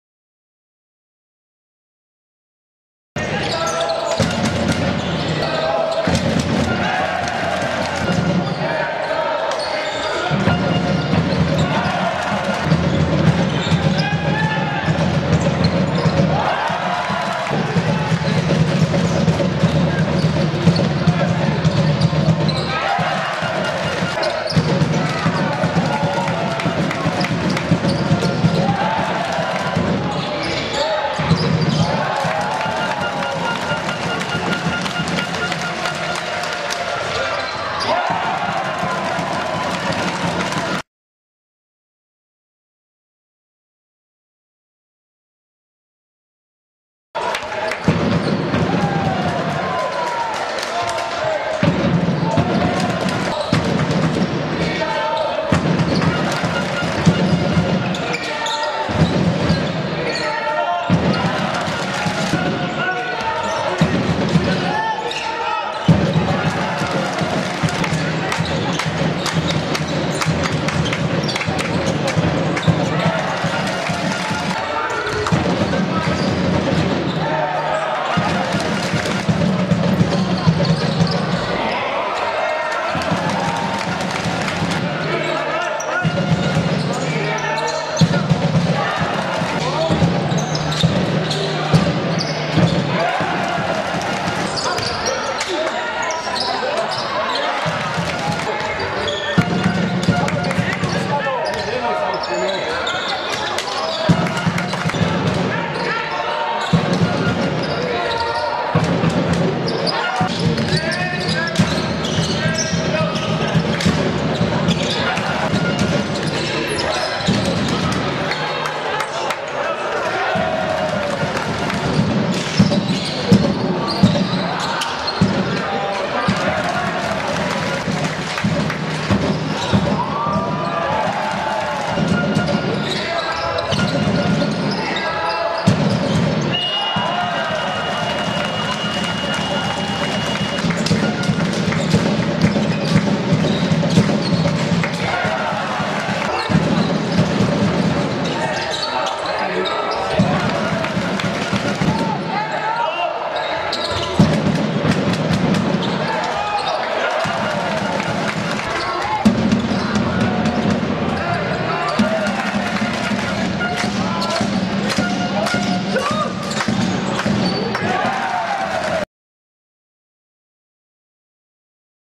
Highlights Langen
Hier die Highlights inklusive Monsterblock von Matt und Yannicks 6 Dreiern in der 1.Halbzeit!!